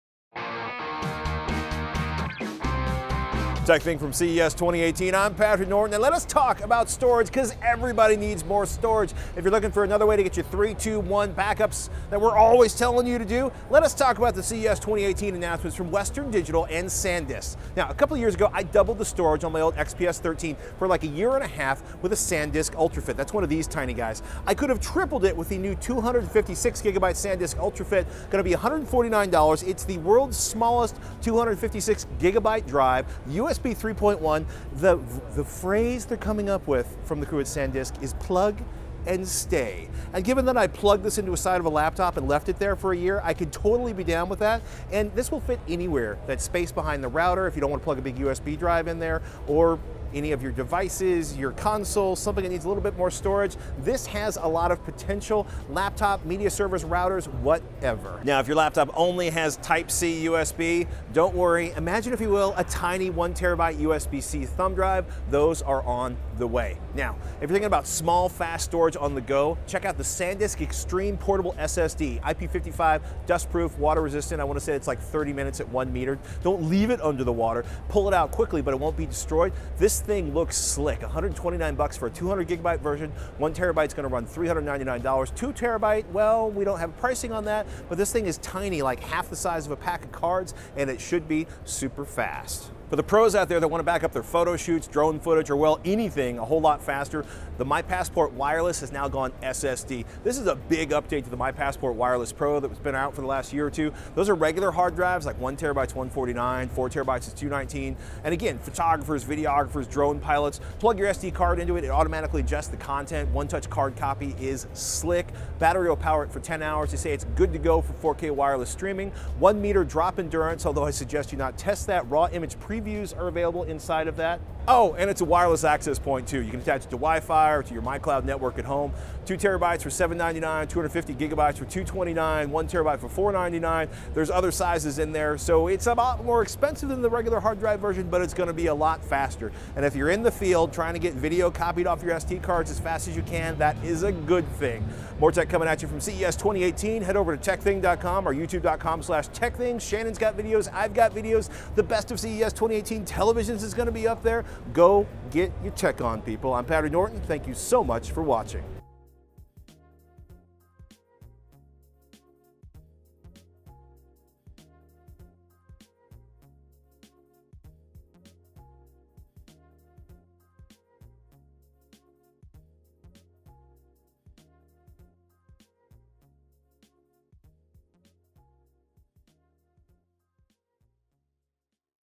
Hands on with the Sandisk 1TB USB-C Flash Drive and Rugged Portable 2TB SSD Announced at CES 2018!
Hands on with the Western Digital Sandisk 1TB USB-C Flash Drive and Rugged Portable 2TB SSD Announced at CES 2018! Plus, new sizes of the UltraFit!

Thank You Patrons! we wouldn't be able to make the show for you every week!

SUPPORT:
Amazon Associates

THANKS!

SOCIAL IT UP!